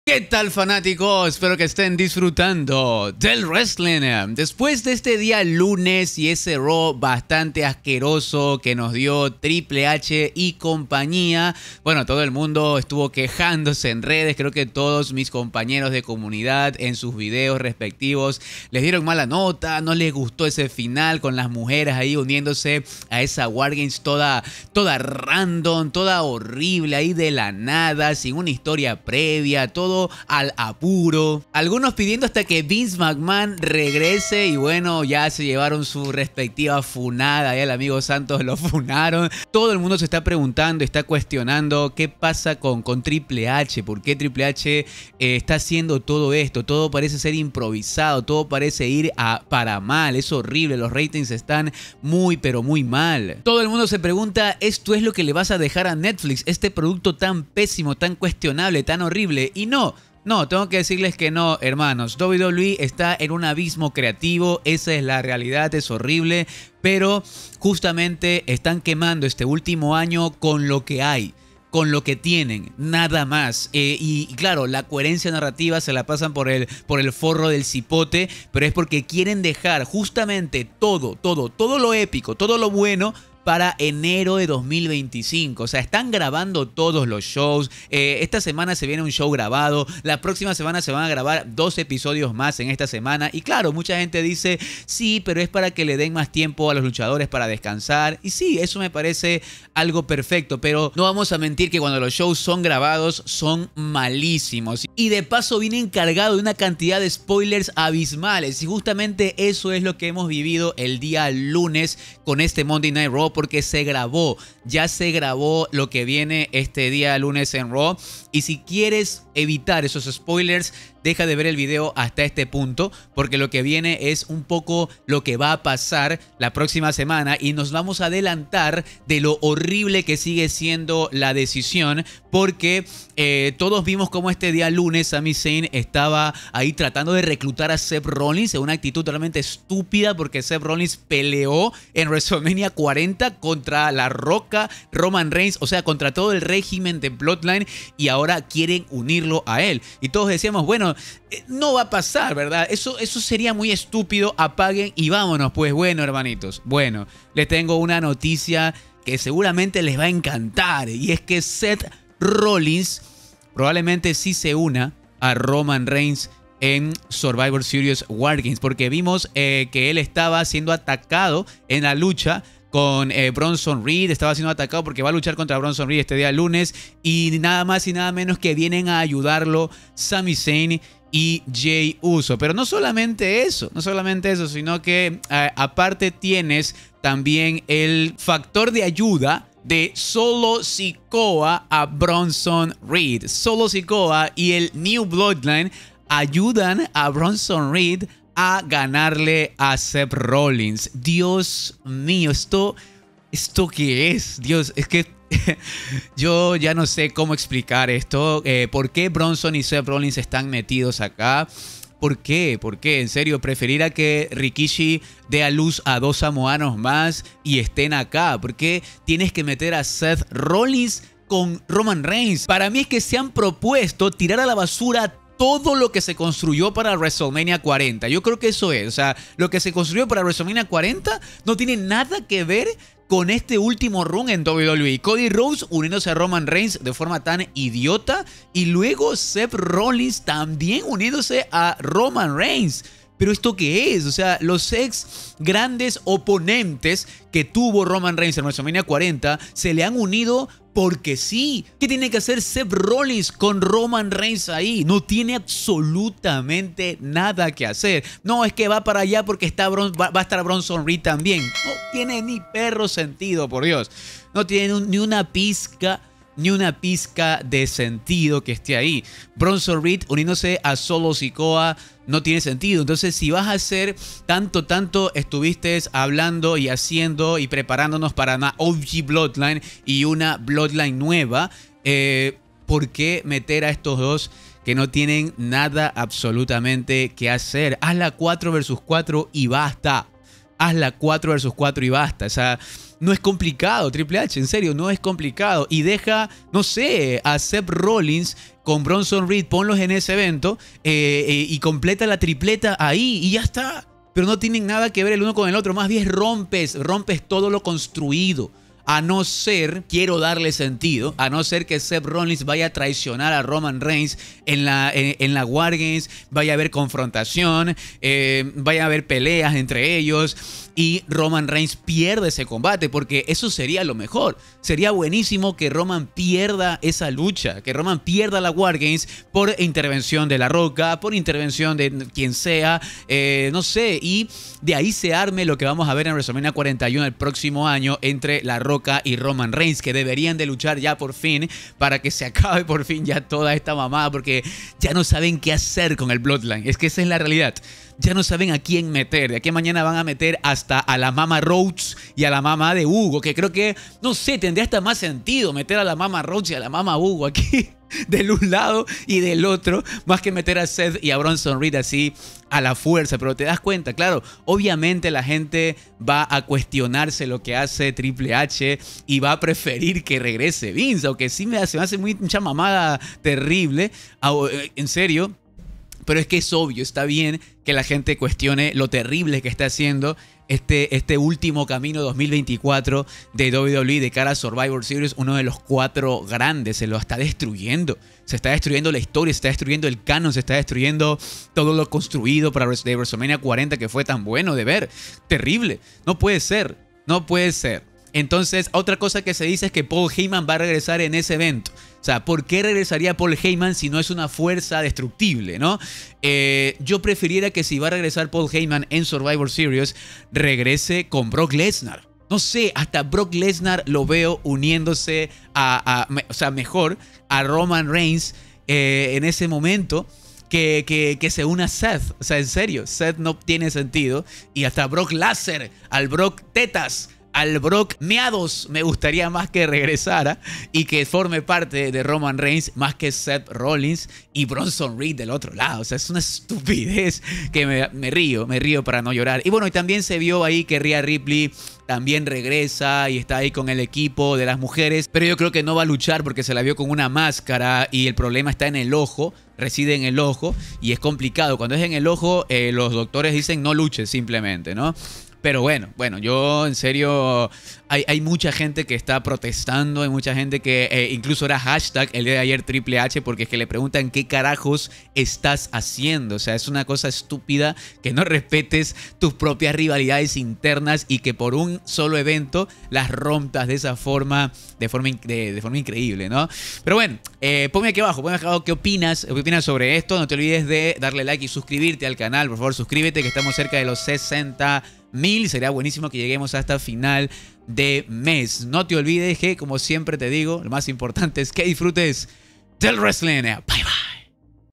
SETH ROLLINS SE UNE a ROMAN REIGNS en WAR GAMES!! 😕 ESTO QUE ES? TRIPLE H PERDIÓ LA CABEZA!!
SETH y BRONSON EN WAR GAMES!!

Conviértete en MIEMBRO y se parte de los SEXY BOYS del Canal 🥰